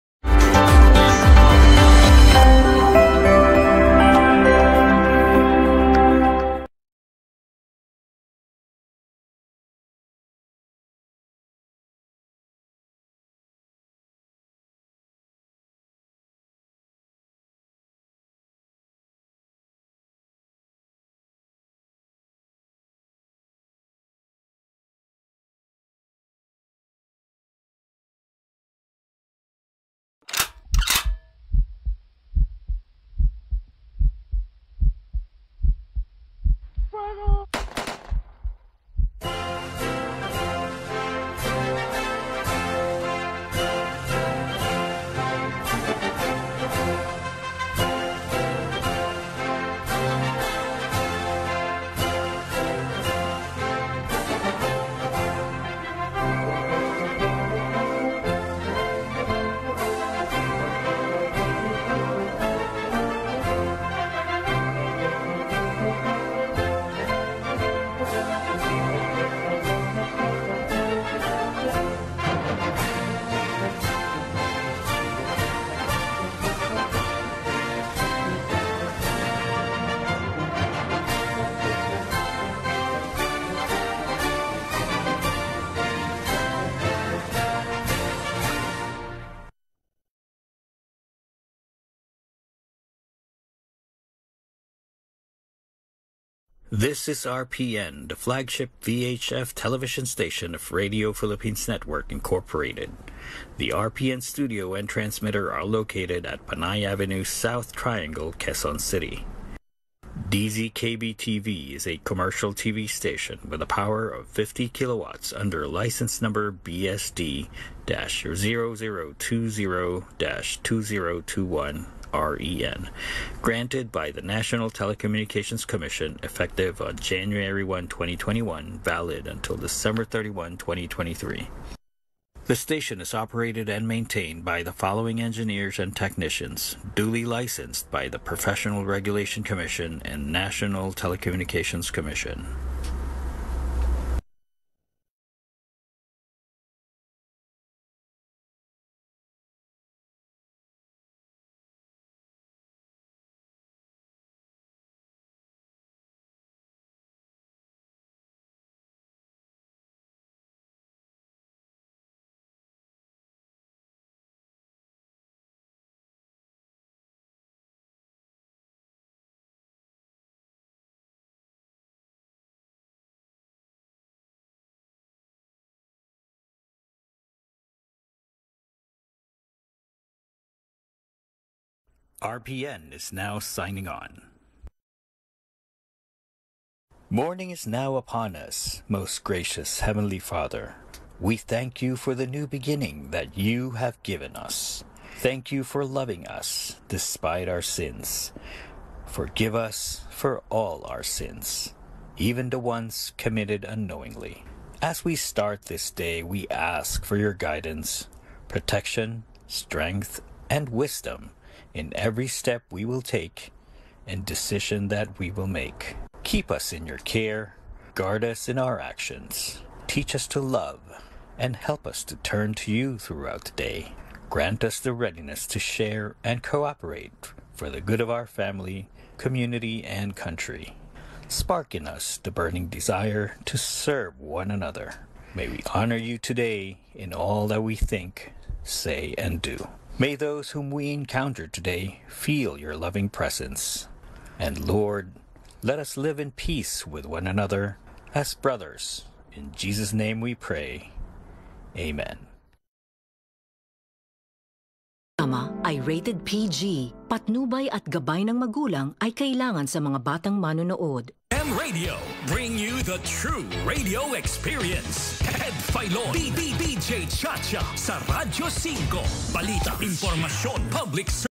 RPTV - first sign-on (5:55am) [1-FEB-2024]
Note: Reuploading or reposting this video is prohibited without the creator's permission.

The first sign-on sequence of RPTV, the new channel replacement for CNN Philippines was recorded on February 1, 2024, at 5:55am. The channel carries selected TV5 programs (including Ted Failon at DJ Chacha sa Radyo5, Eat Bulaga!, PBA games, alongside others) alongside classic RPN shows. The sign-on sequence uses RPN 2022 sign-on video, first aired exclusively on analog since November 2022.